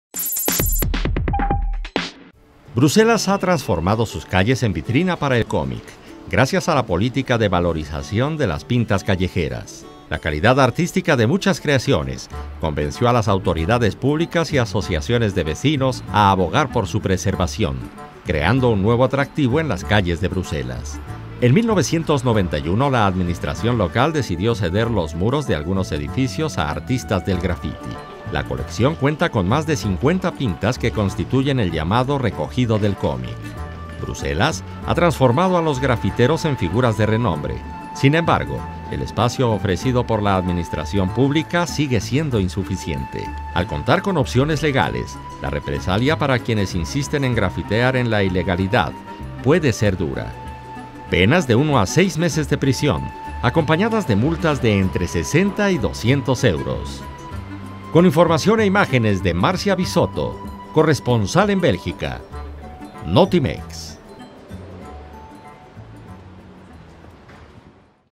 Florecen en Bruselas proyectos de grafiti legal
Bruselas, ha transformado sus calles en vitrina para el cómic